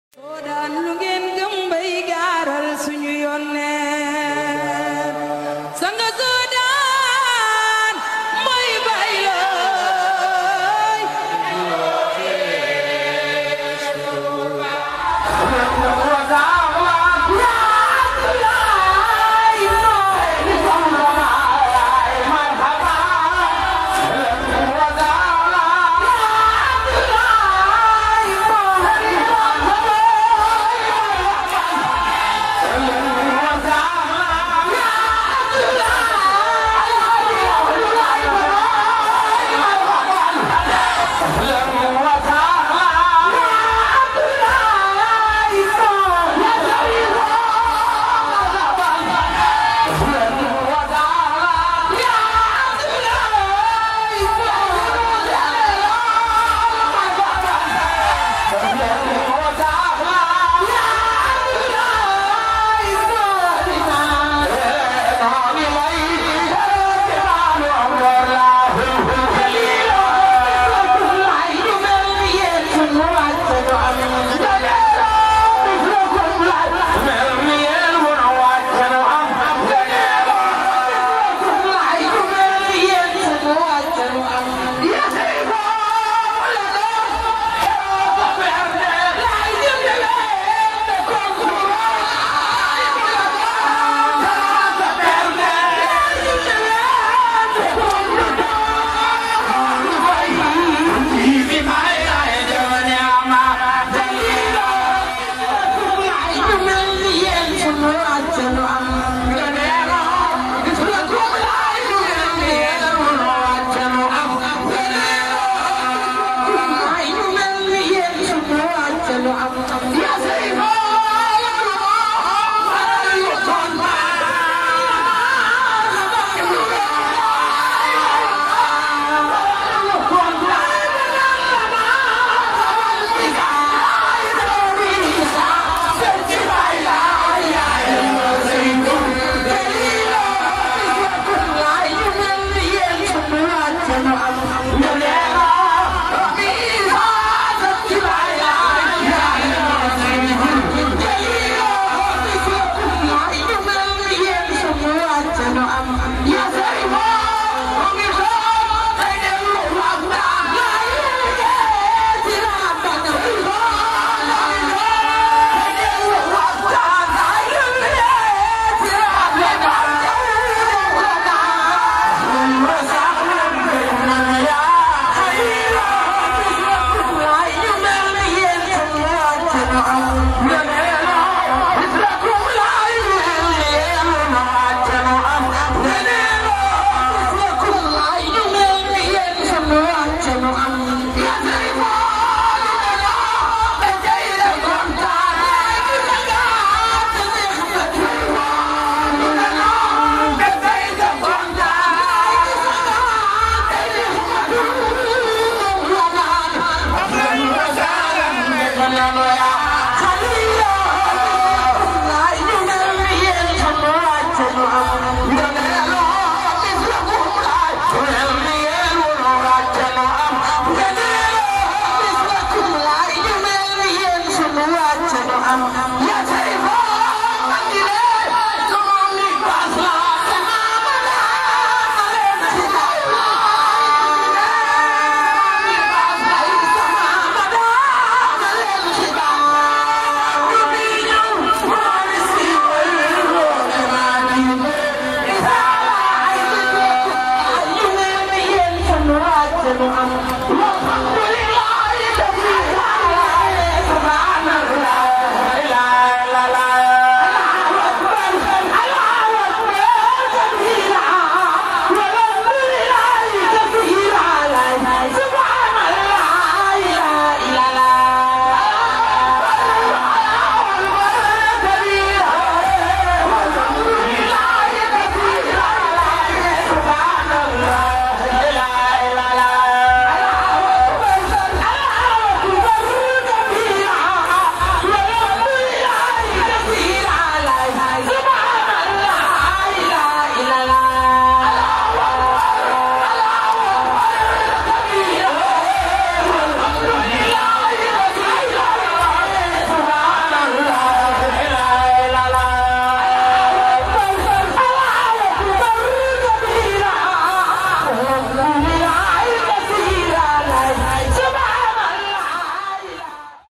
JANGUE THIES 2019 : Ahlann wa Sahlann
Les chants religieux de Thiès ont démarré par l'arrivée de Chérif Baye Ndjine Thiaw LAHI à la Promenade des Thiessois. Voici comment il a été accueilli.

Elle est disponible via l'application Android du même nom ou via son site internet ou la page facebook du journal Waa SOODAAN...